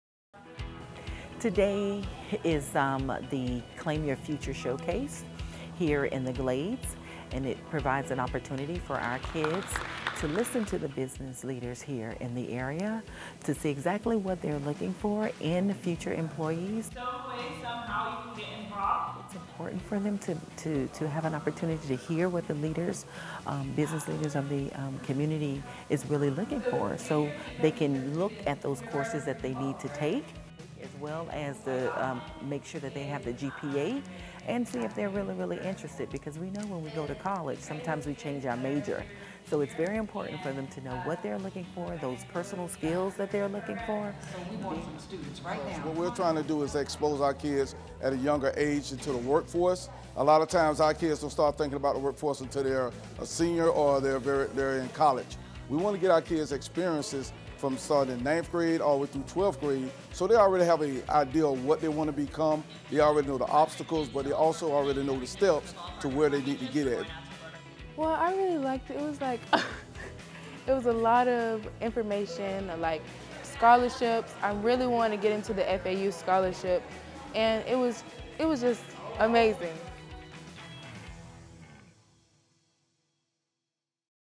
Glades Claim Your Future Showcase (2020)
Local businesses come together in the Glades to show high school students the possible career paths available in Palm Beach County.